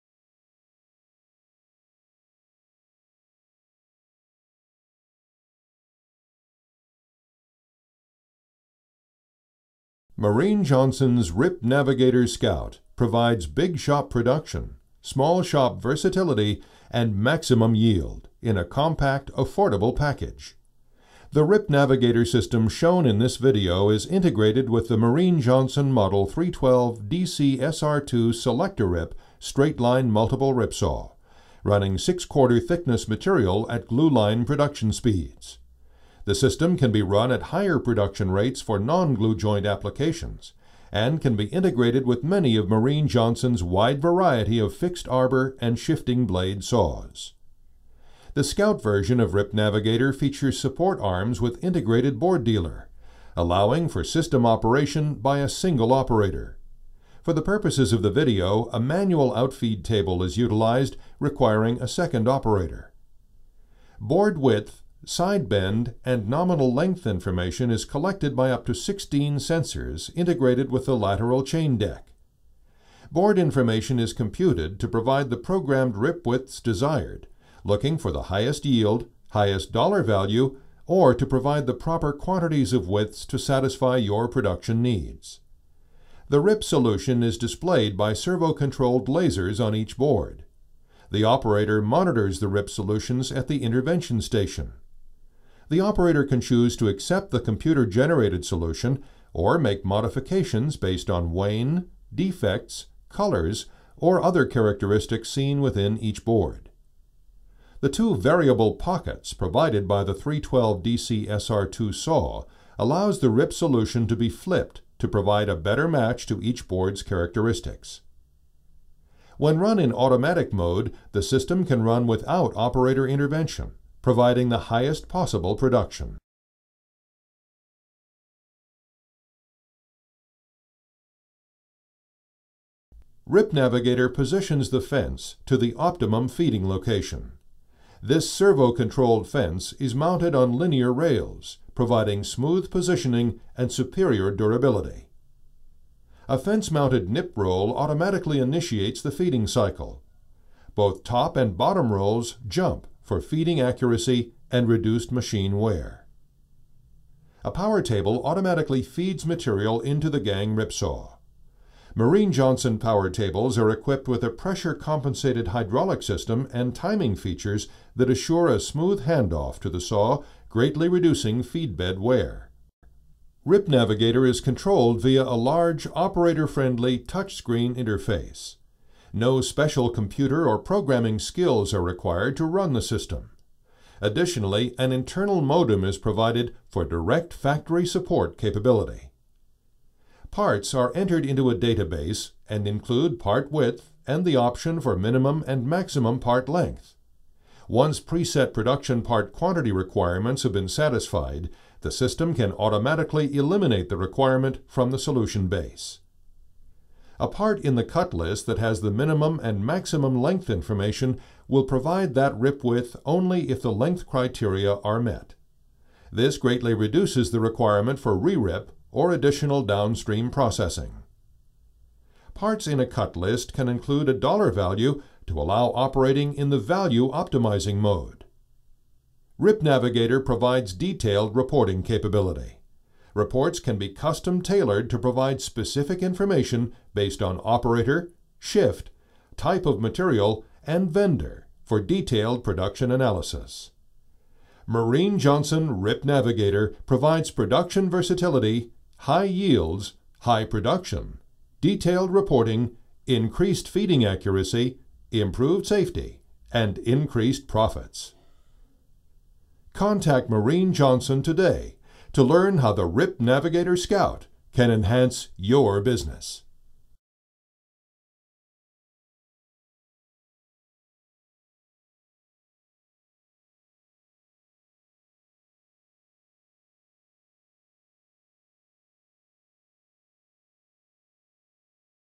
Mereen-Johnson Rip Navigator Optimizing System
Rip Navigator Scout Optimizing System
Mereen-Johnson Machine Companys Rip Navigator Optimizing Scout System for the Gang Rip Saw provides big shop production and features in a small footprint and is affordably priced.
Rip Navigator is equipped with a multiple point measurement system to define board width, length and crook.  The system can be run in automatic mode or can be monitored by an operator to evaluate each computer generated cut solution.
Rip Navigator provides increased production, better feeding accuracy, detailed production reporting, improved lumber yield, higher profits and greater operator safety.